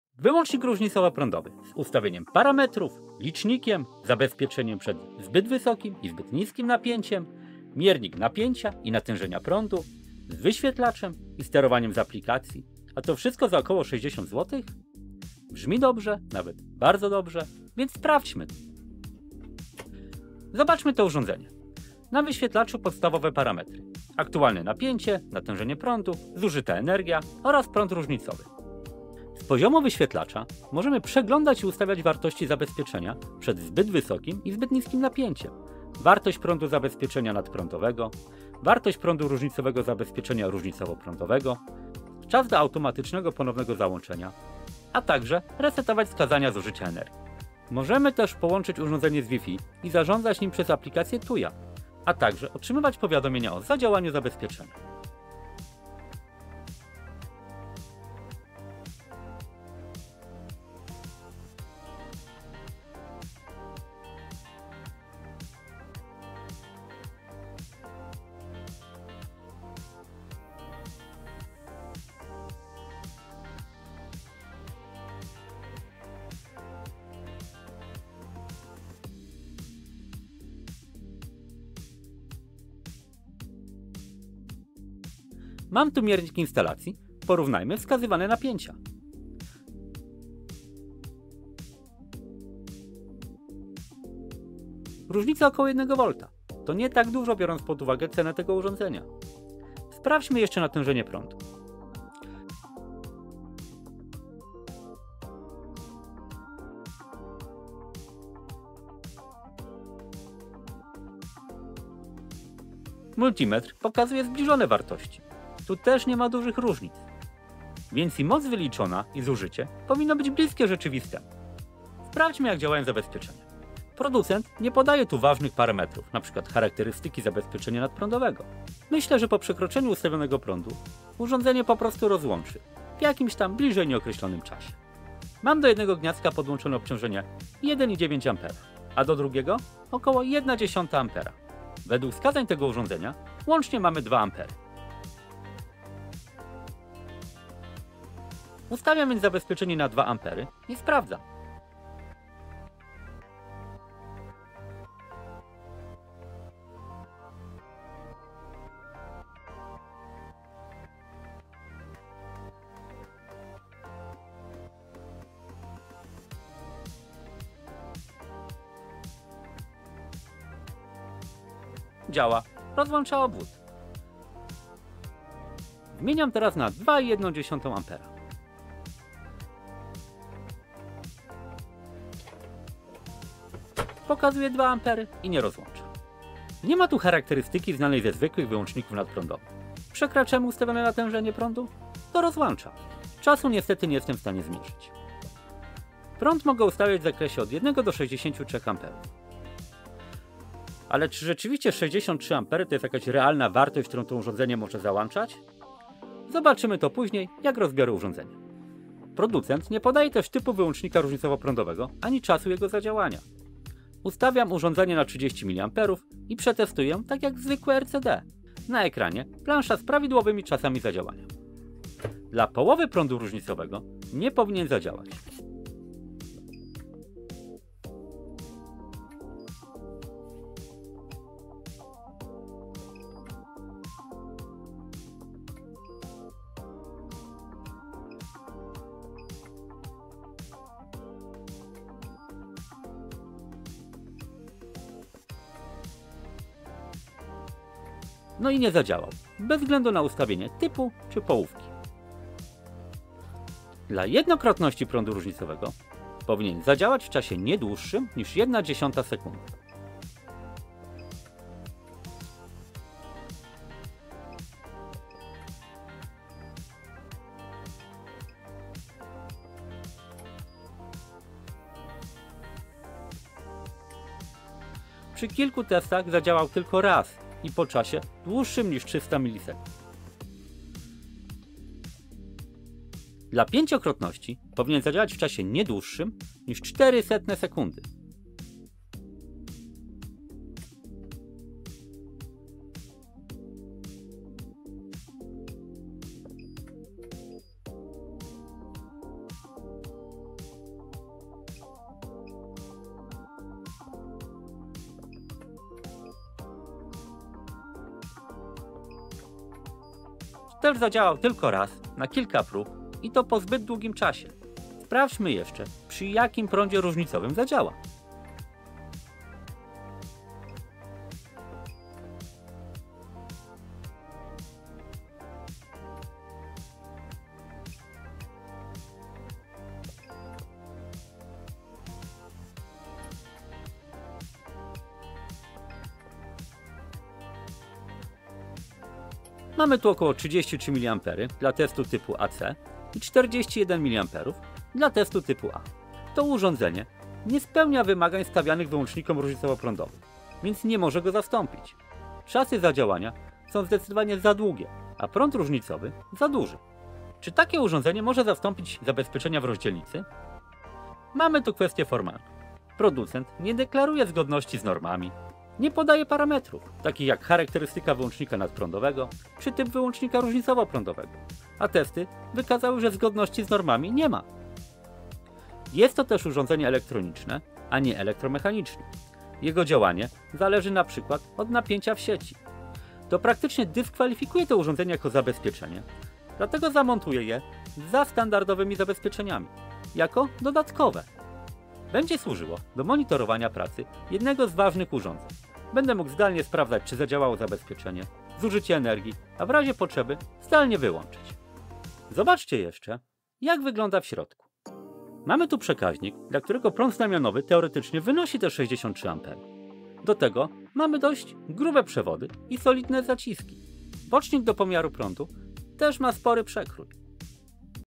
Genialne urządzenie zamiast zabezpieczeń?
Wyłącznik nadprądowy i różnicowoprądowy, z ustawieniem parametrów, licznikiem, zabezpieczeniem przez zbyt wysokim i niskim napięciem, miernik napięcia i prądu, z wyświetlaczem i sterowaniem z aplikacji - a to wszystko za około 60 zł?
Brzmi dobrze, nawet bardzo dobrze, aż za dobrze wręcz niewiarygodnie, wiec sprawdźmy to.